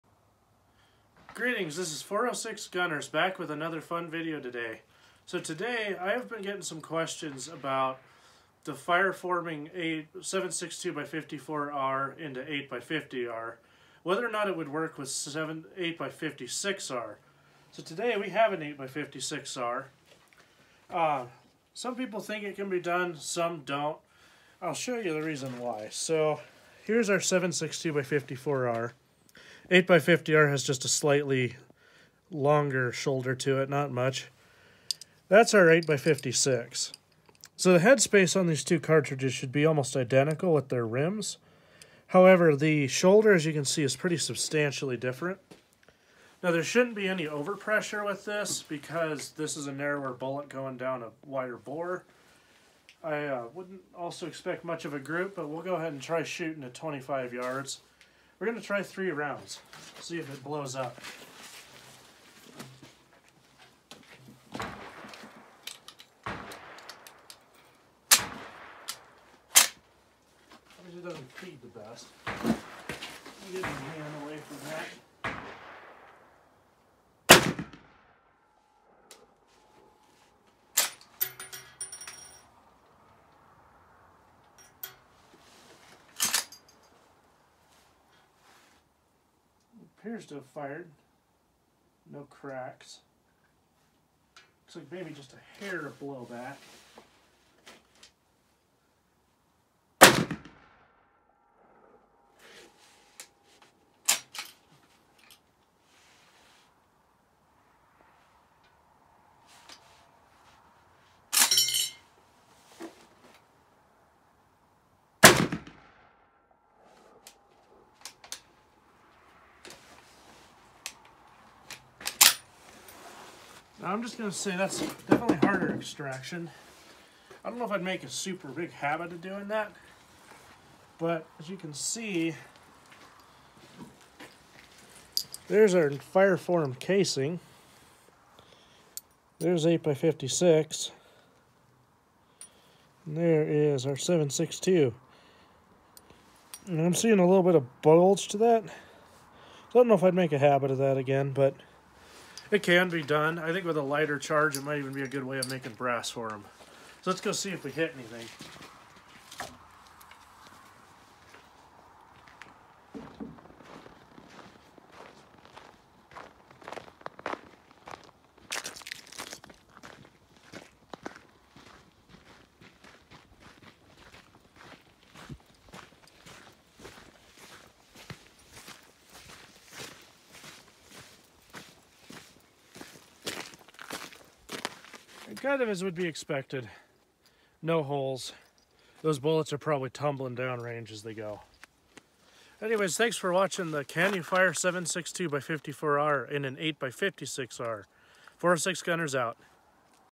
Shooting 7.62x54r in a 8x56R Mannlicher M95
We fire 7.62x54r in a mannlicher M95 In 8x56r

Please Do NOT attempt with steel case ammo.